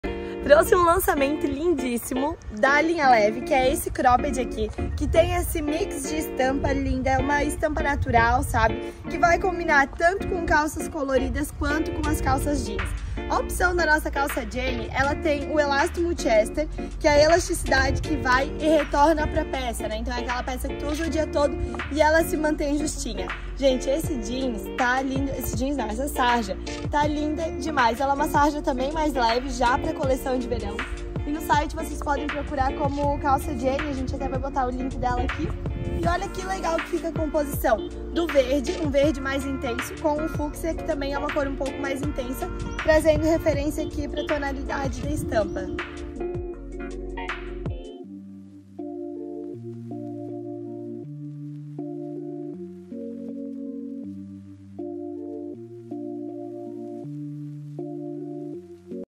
Calça Jeans Collor Jheny Feminina Cosh
- Novo modelo de Calça Jeans Collor da Cosh Jeans. Um modelo belissimo, com cores elegantes e que são tedência para o verão 2023. Nossa calça jeans collor Jheny modela perfeitamente no corpo, confeccionada em jeans de alta qualidade, com aviamentos super estilosos e modelagem exclusiva.

Nossa coleção de verão está recheada de modelos belissimos para você copor looks superestilosos.

Corre garantir seu look aqui na Cosh Jeans